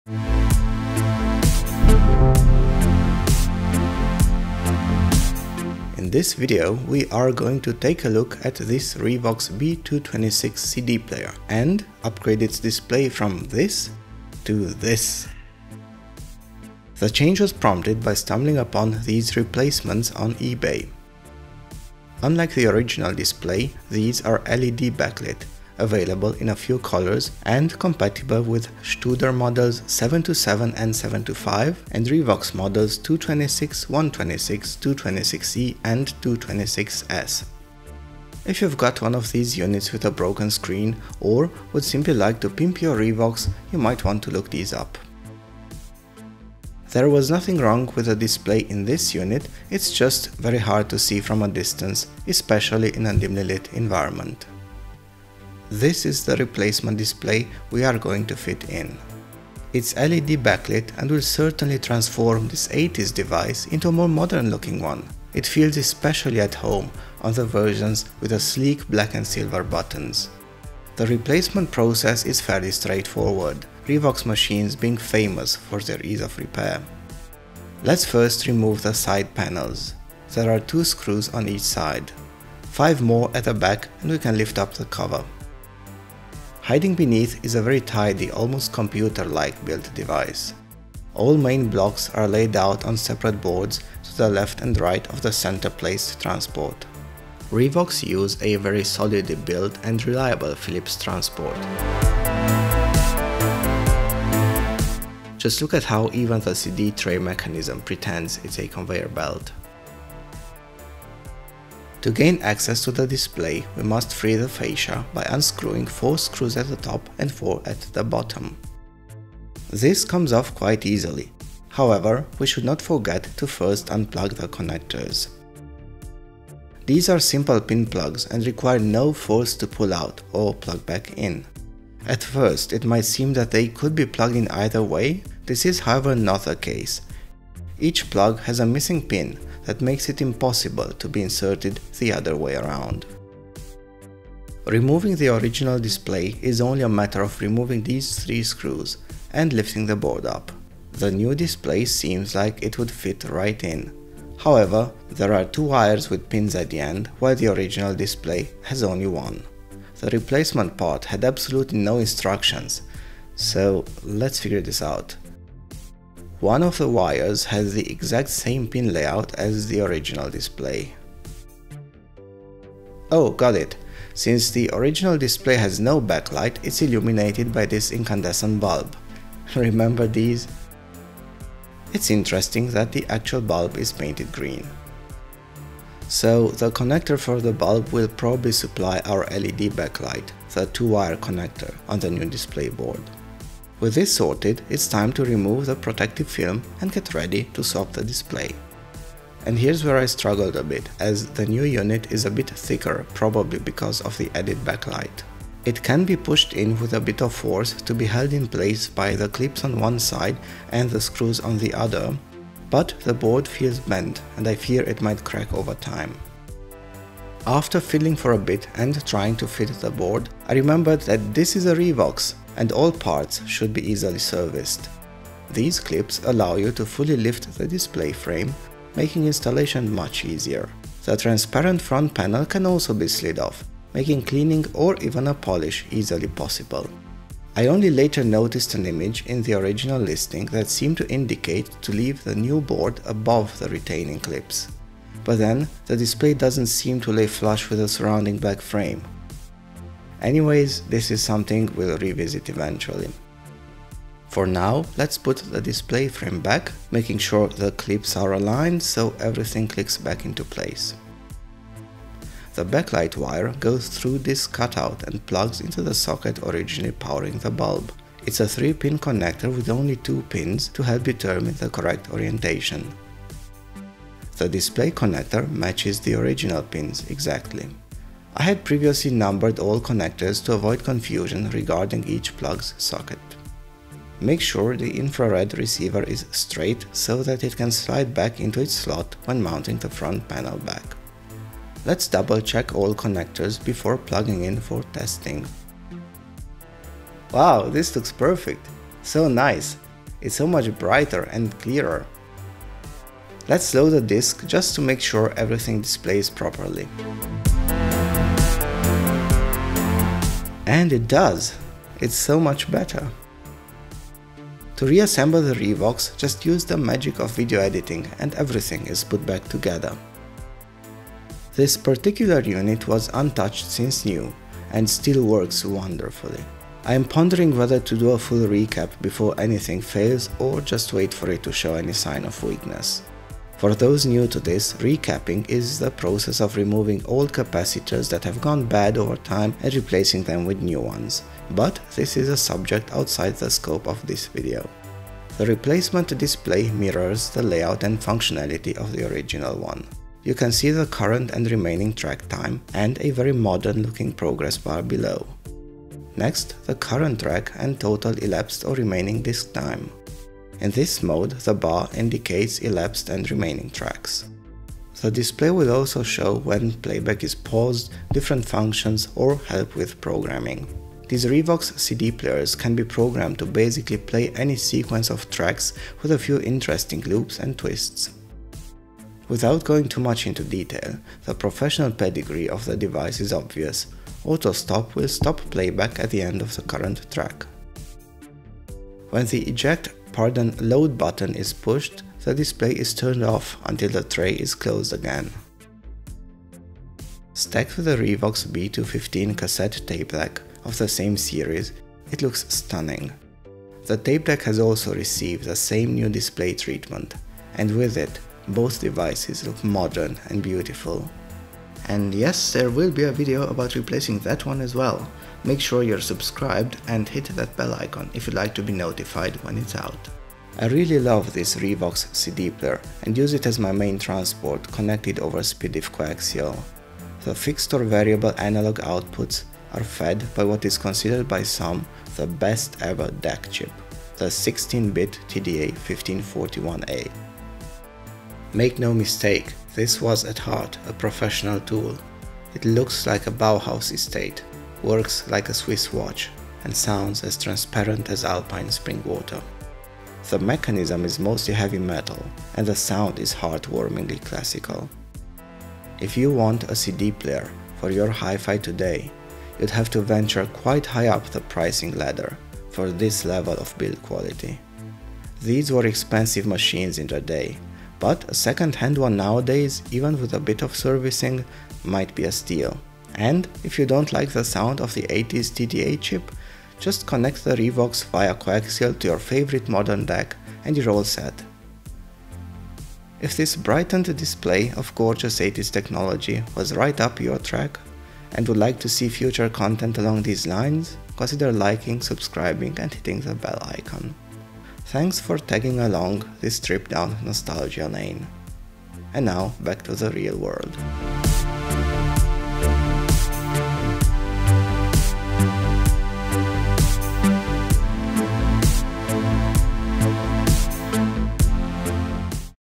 REVOX B226 CD Player • Display Replacement and Review (feat. B215 Cassette Tape Deck)
A look at the Revox B226 CD Player while we modernise the display from the original 80’s LCD to a new, LED backlit display. This is a tutorial and a mini-review as we touch upon different features of this truly stunning device.

Featuring a guest appearance of the B215 Cassette Tape Deck.

Your support will really make a difference in how often these complex videos can be made.

0:00 Intro
0:17 The new Display - Info & Compatibility
0:46 Ok, but why?
1:11 Tutorial - Taking the unit apart
2:24 Tutorial - Swapping the Display
4:51 Testing
5:45 Display Features
6:37 Revox B226 CD & B215 Cassette Deck
7:00 Mini-review & Conclusion
8:03 Outro